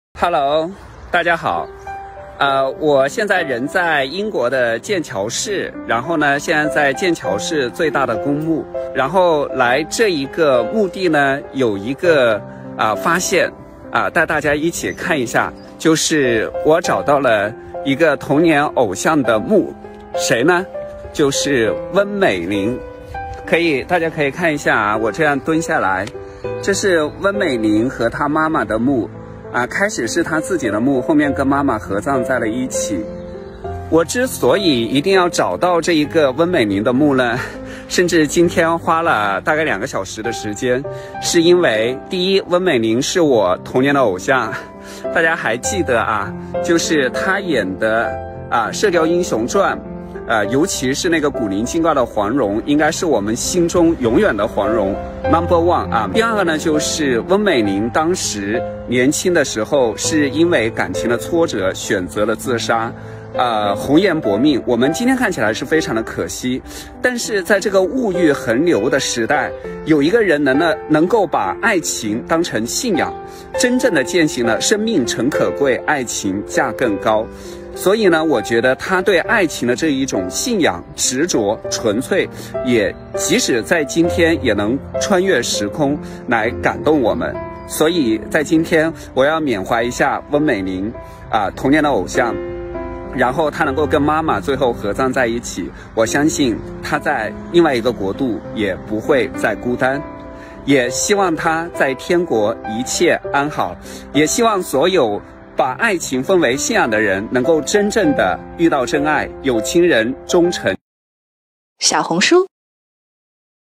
在剑桥遇见翁美玲墓。怀念永远年轻的童年女神,永远的黄蓉!
陈历杰(Jack),亲密关系咨询师

翁美玲 黄蓉 在剑桥遇见翁美玲墓。怀念翁美玲,永远年轻的童年女神,永远的黄蓉!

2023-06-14

83版